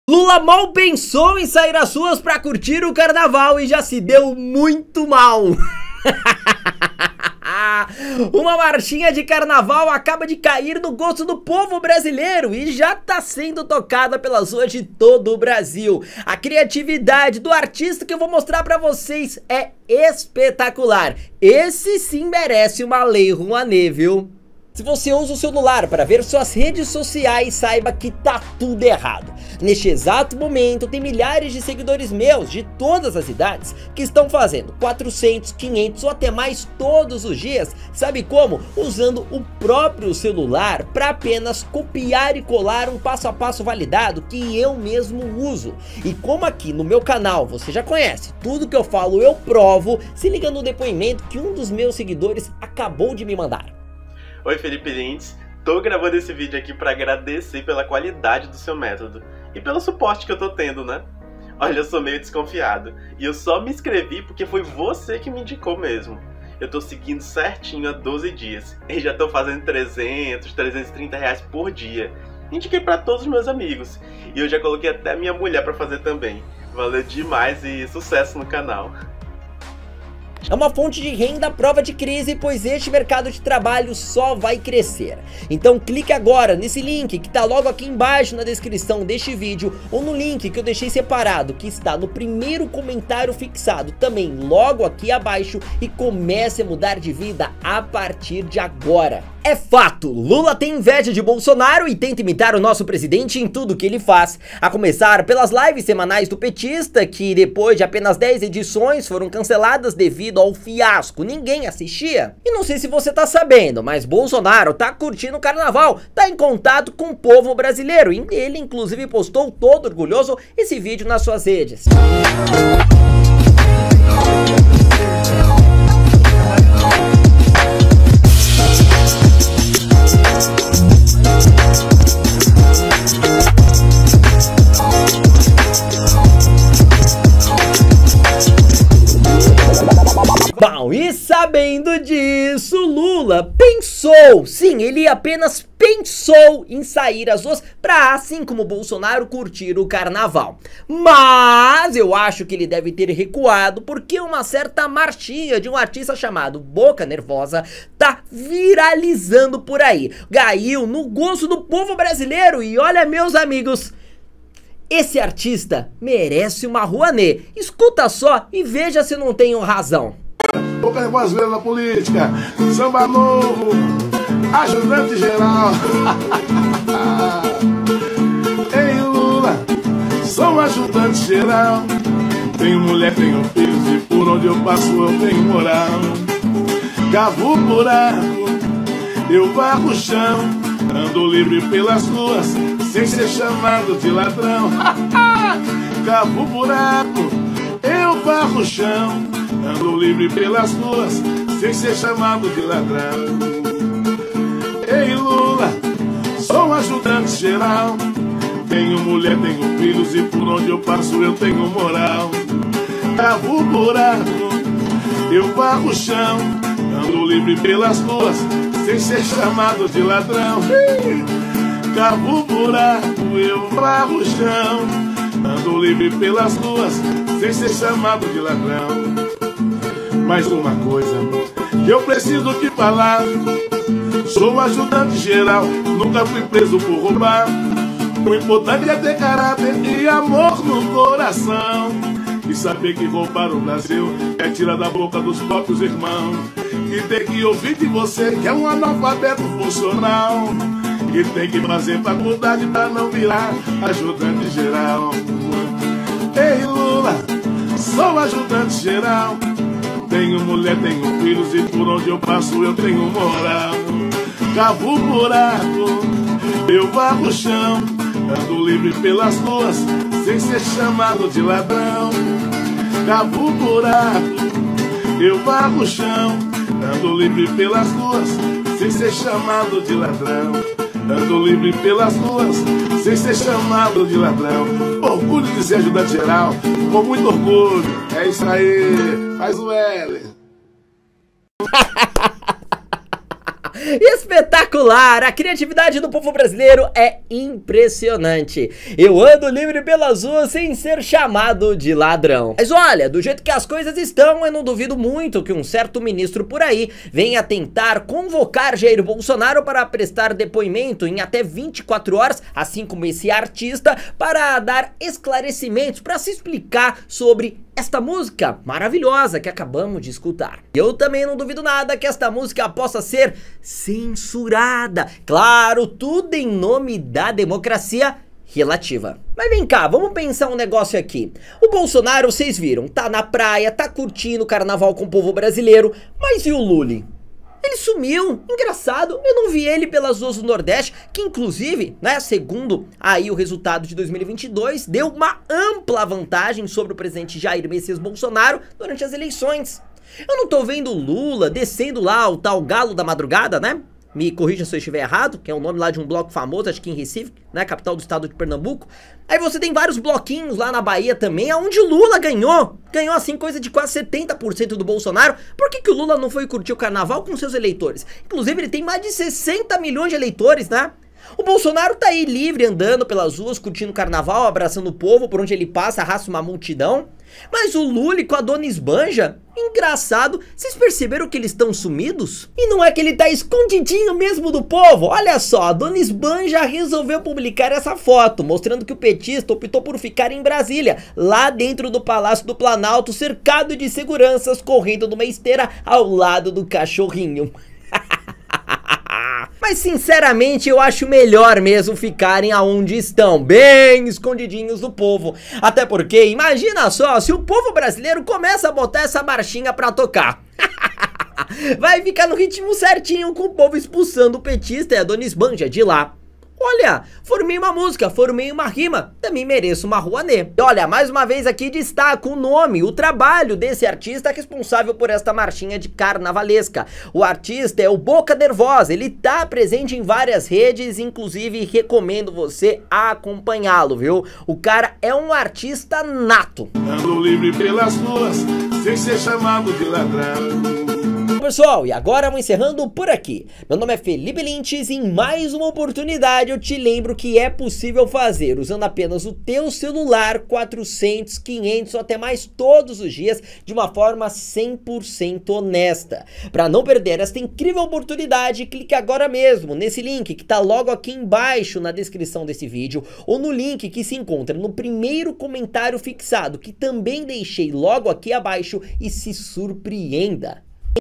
DEU RUIM: Lula e Janja são EXPULSOS do carnaval e correm pra se esconder do povo!
☝🤑 CIique no Iink para Fazer a InteIigência ArtificiaI TrabaIhar para Você e Ganhar DlNHElR0 💵 de Verdade na lnternet!